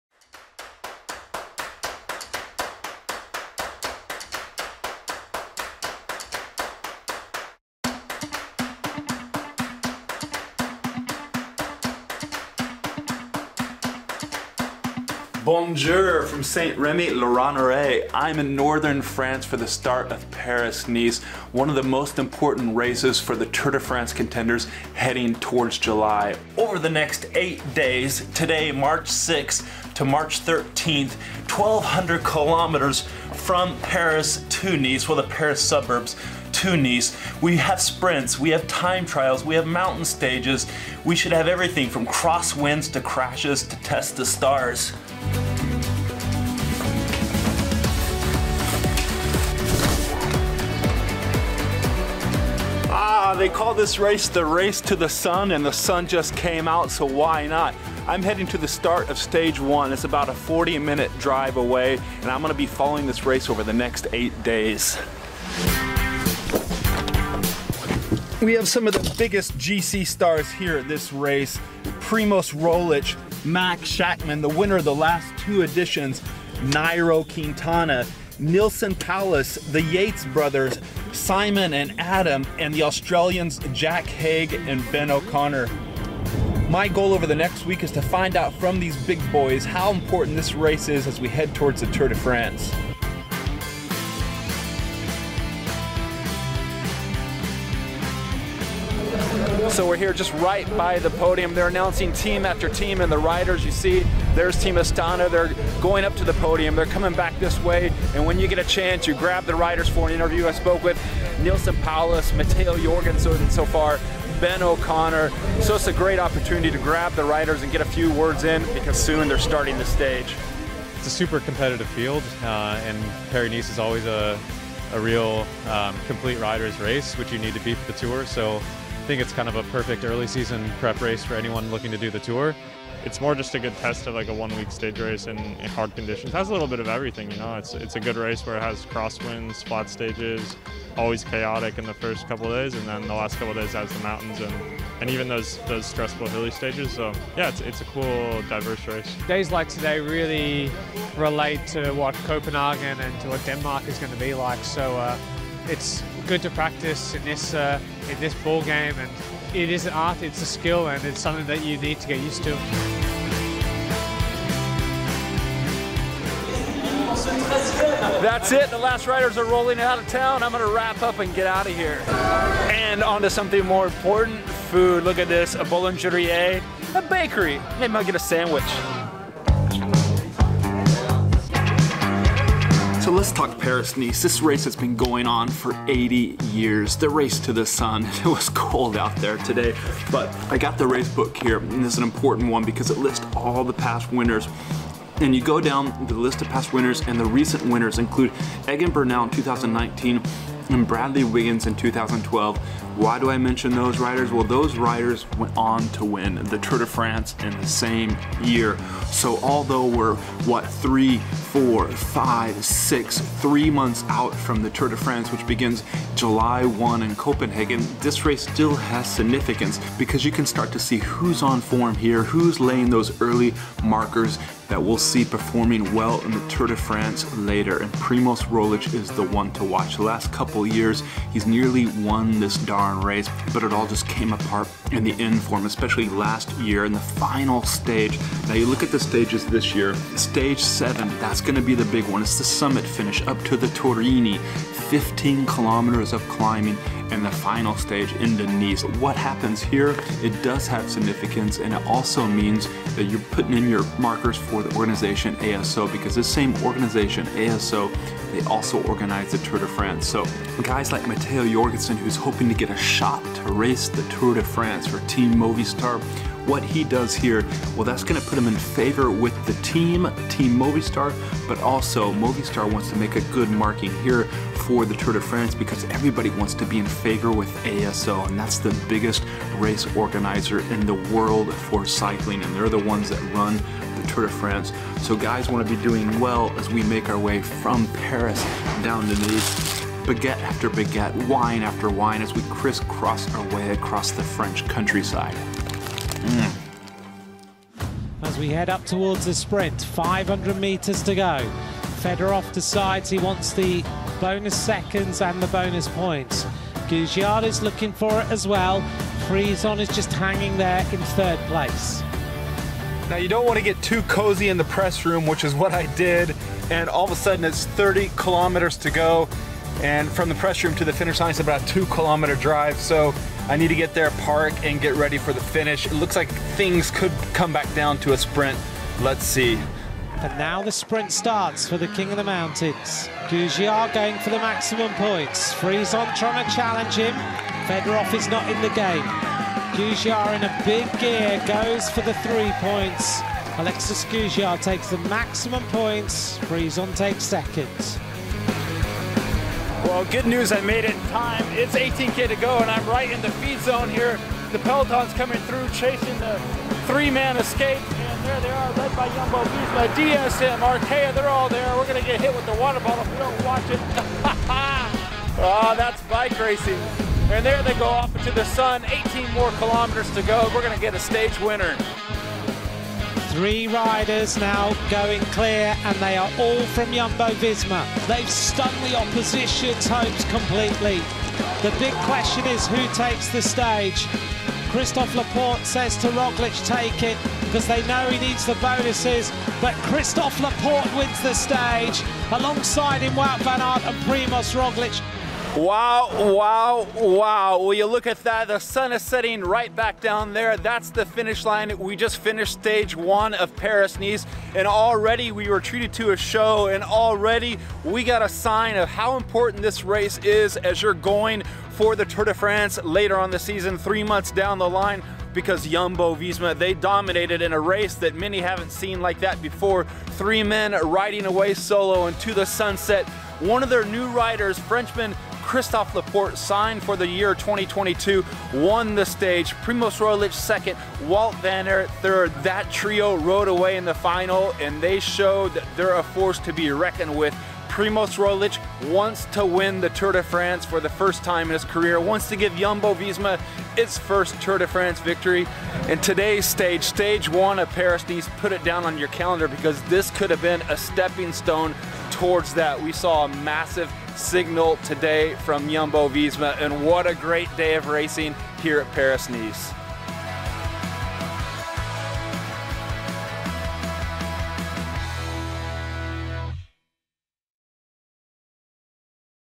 Paris-Nice Is Revealing The World's Strongest Team
Follow Gregor Brown of FloBikes behind the scenes as he works the first big stage race of the 2022 season, Paris-Nice.